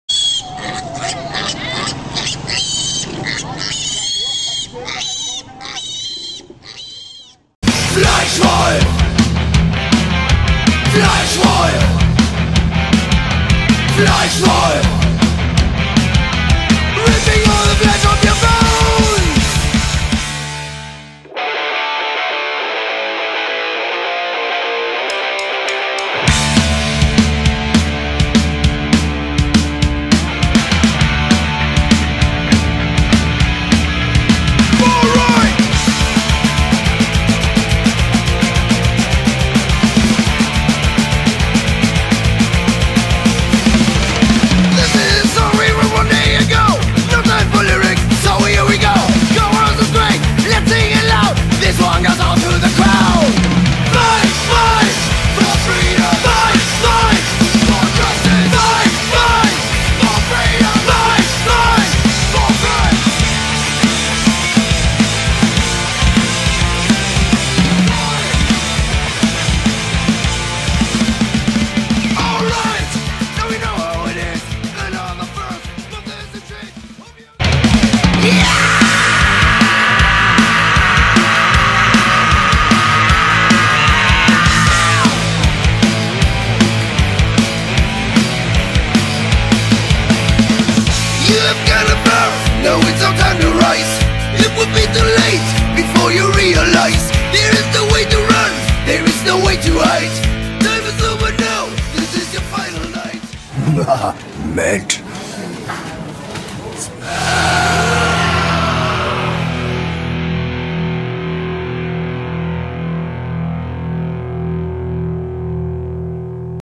EP 2016 Teaser
Bald isses soweit, dann lassen wir unsere ersten 6 Tracks auf euch los. Hier schonma ein kleiner Vorgeschmack.
Slay tuned!
FLEISCHWOLF